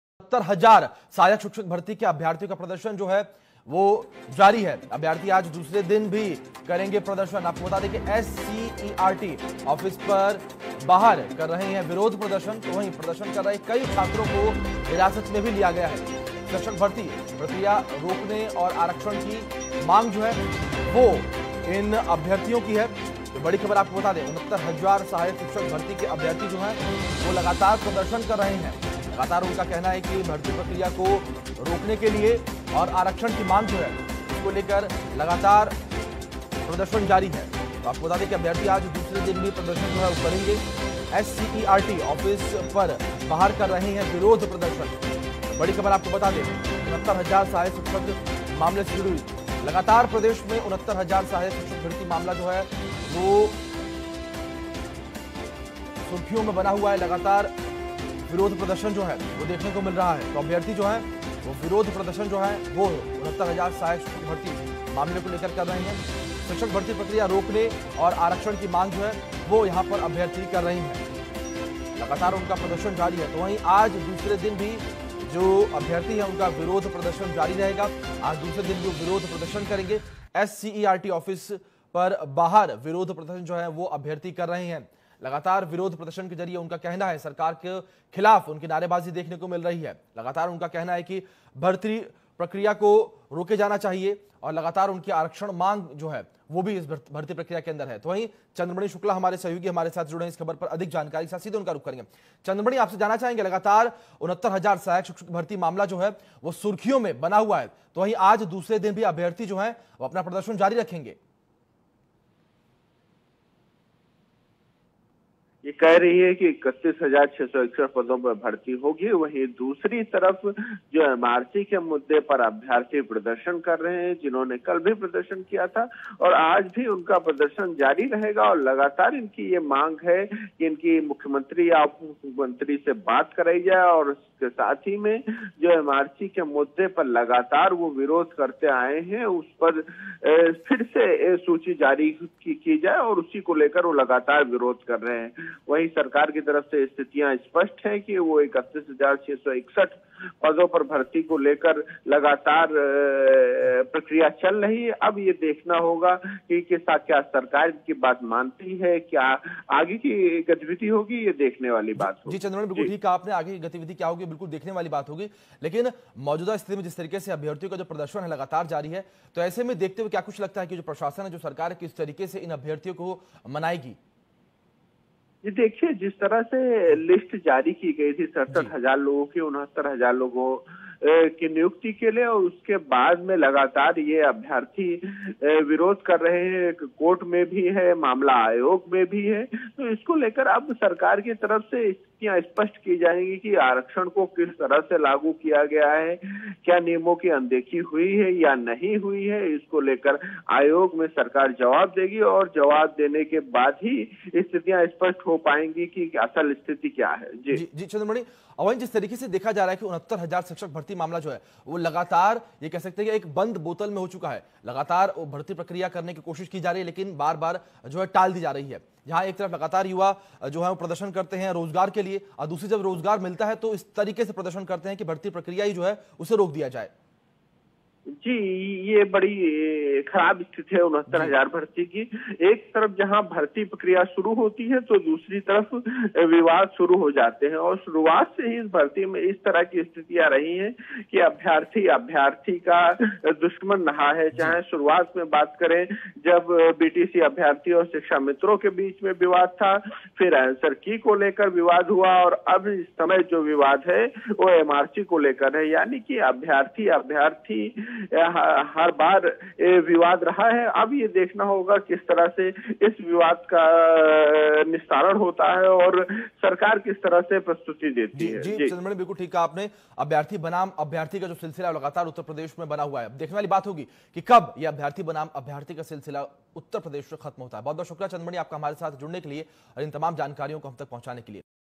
69 हजार सहायक शिक्षक भर्ती के अभ्यर्थियों का प्रदर्शन जारी, अभ्यर्थी आज दूसरे दिन भी करेंगे प्रदर्शन
69000 Shikshak Bharti breaking news
69000 Shikshak Bharti latest news
Shikshak Bharti breaking news